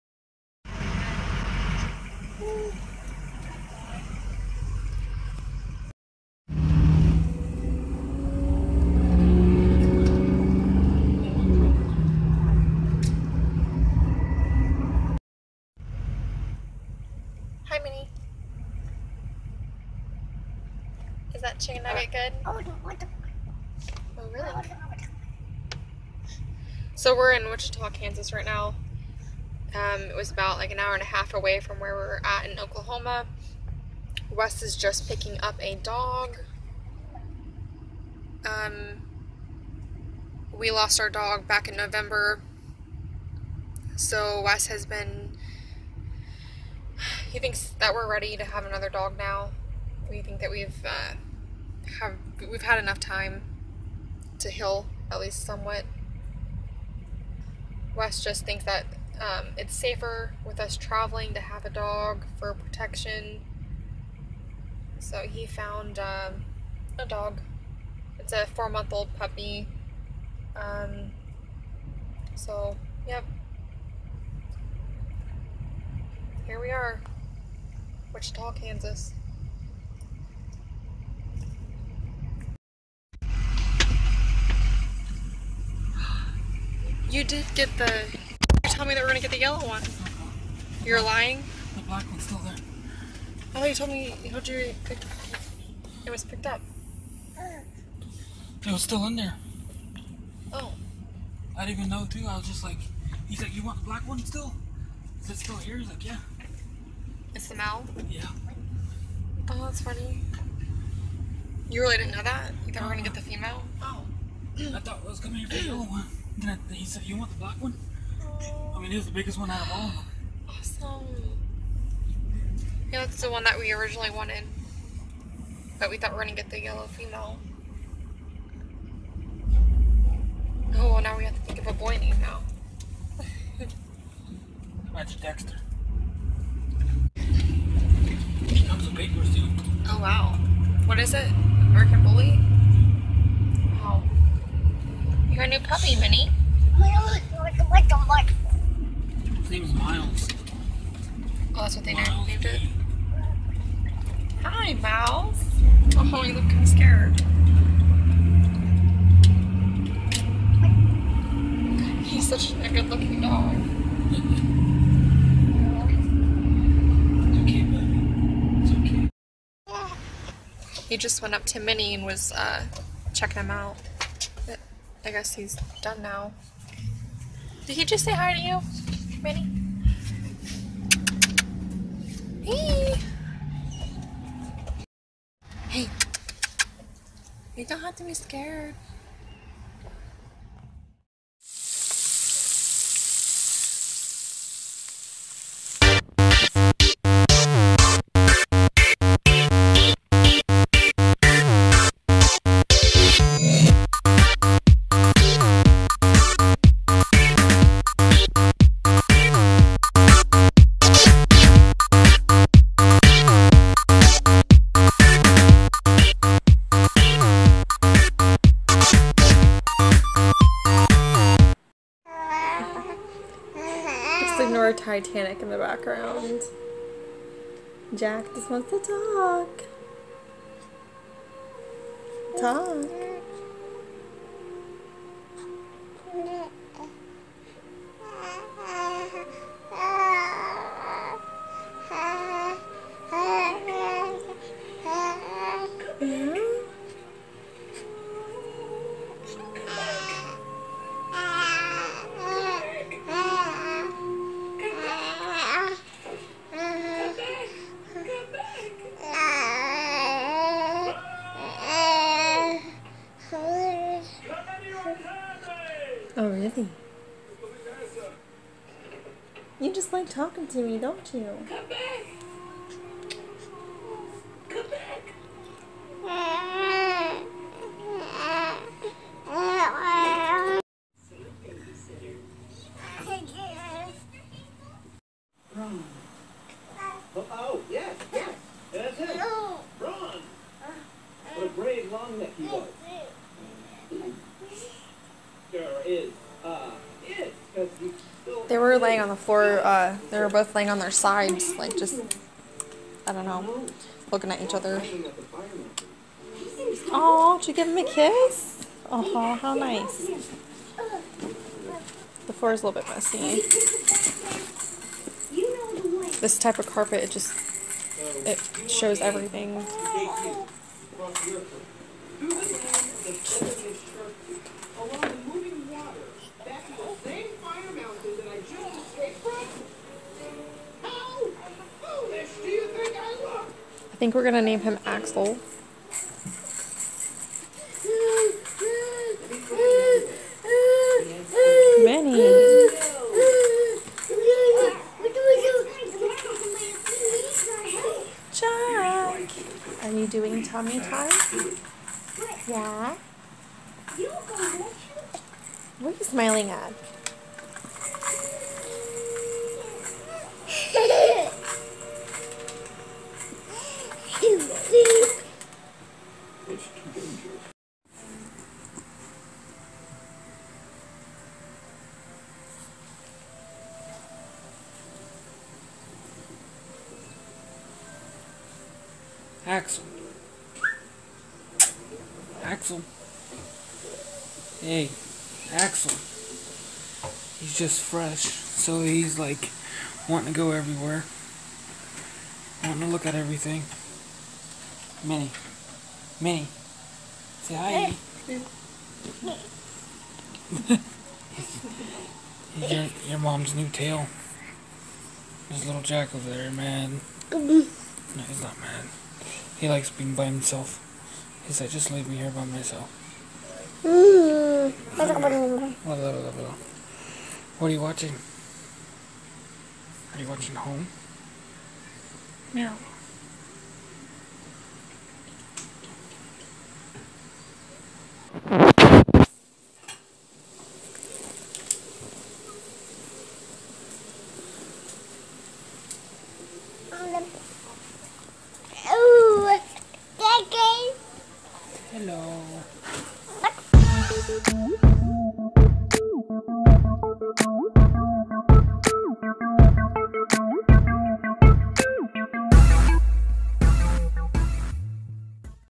Meet our new dog! American Bully! Family road trip to Wichita Kansas.
Roadfamily vlog
We went to Wichita Kansas to pick up our new dog Axel. He is a 4 month old American Bully. We recently lost our dog Arthur who passed away on February 1st. He will be missed and we will never forget him. We are now ready to love our new dog Axel.
This video has Subtitles for the deaf.
There is also some footage of me making vegan Ramen.